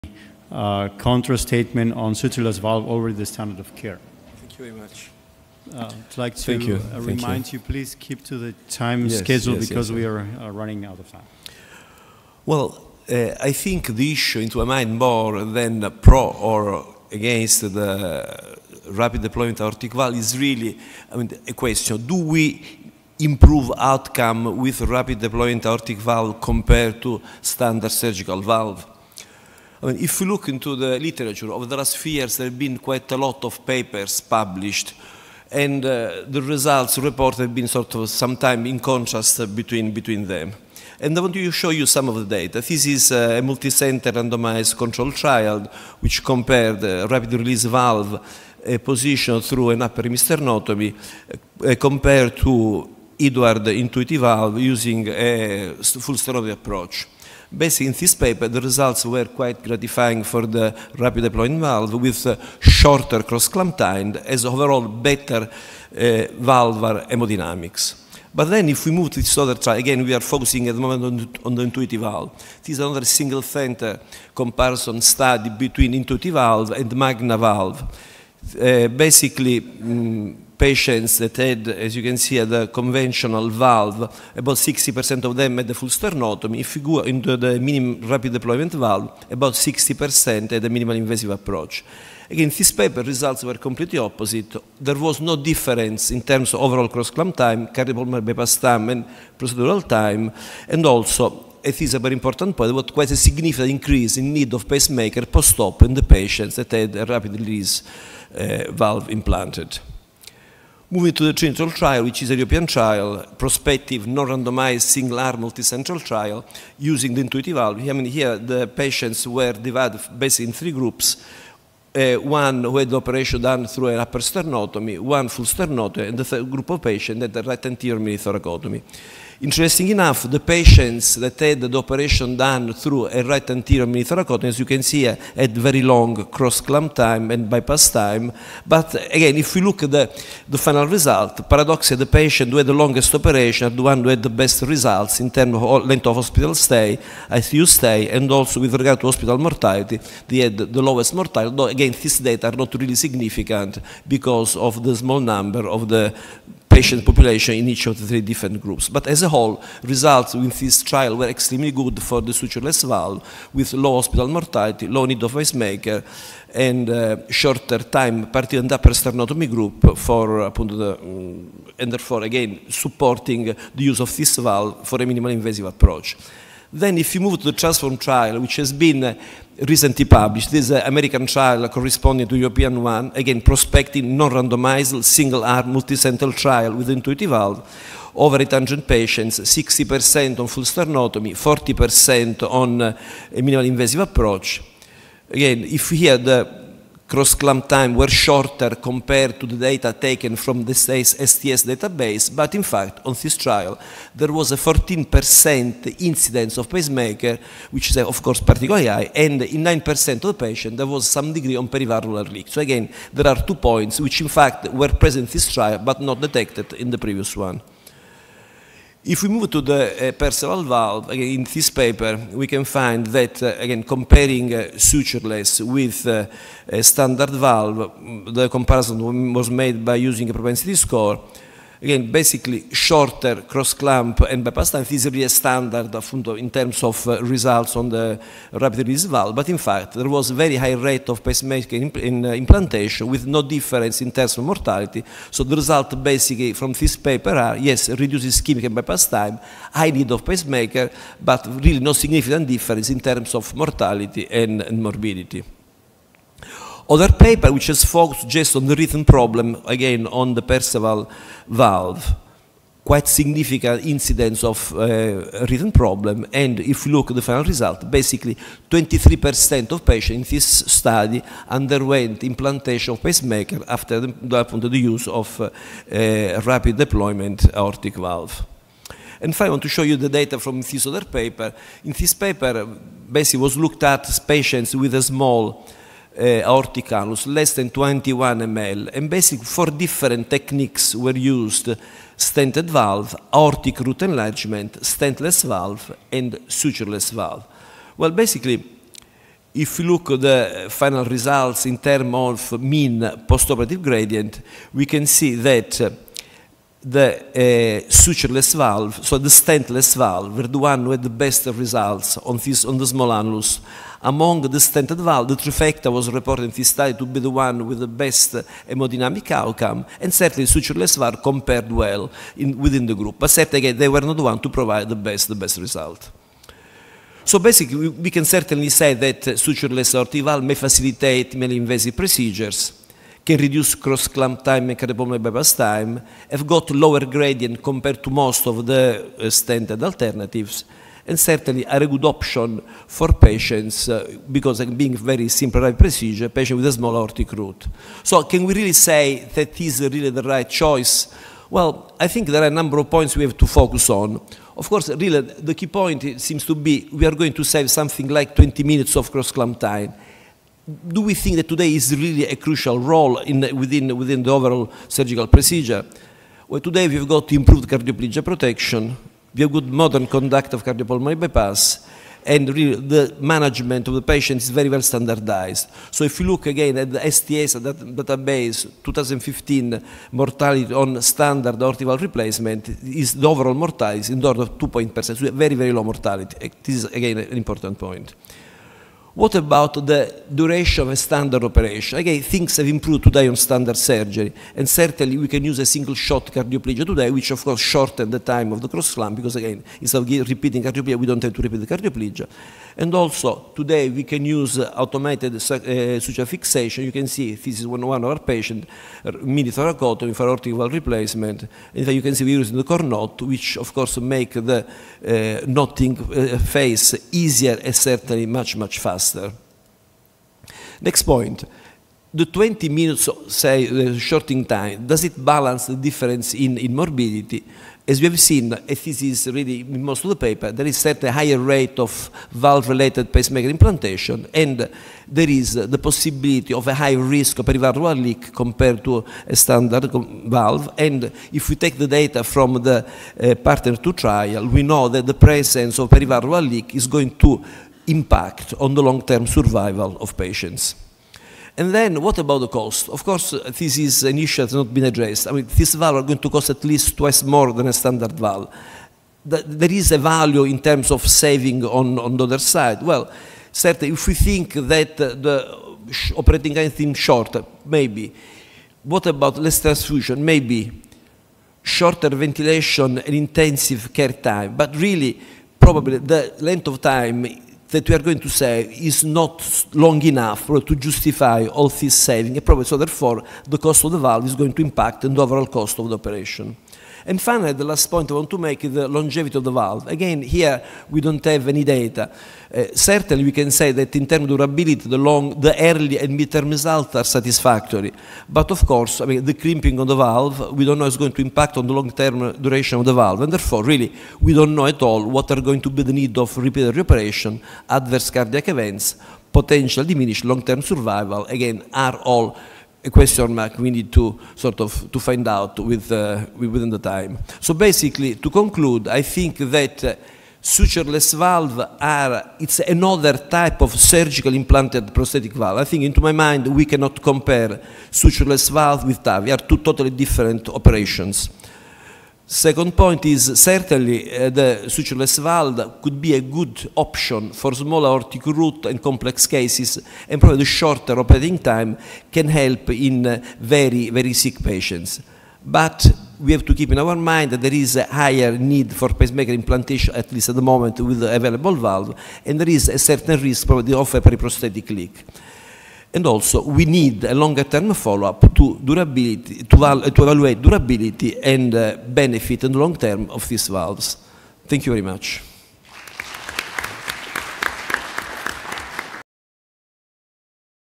14.10-14.20: Sutureless AVR after Failed TAVR, Mattia Glauber, MD
Wednesday, 7 June 2017, Masters Day, 2017 ISMICS Annual Scientific Meeting, Rome Cavalieri, Rome, Italy, 7-10 June 2017, Sutureless AVR after Failed TAVR, Annual Scientific Meeting, ISMICS, International Society for Minimally Invasive Cardiothoracic Surgery